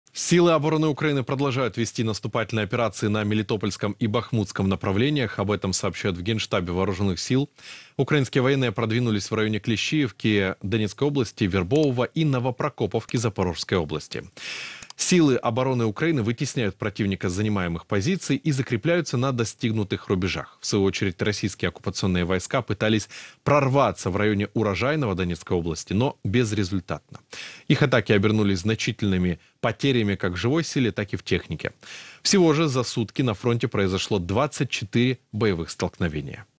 ВСУ продвигаются в районе Клещиевки! Как проходит наступление Украины?
ВСУ продолжают вести наступательные операции. На каком направлении украинские защитники достигли успеха? Где ВСУ удается продвигаться вперед? Какая сейчас ситуация на фронтах? Детальнее в сюжете.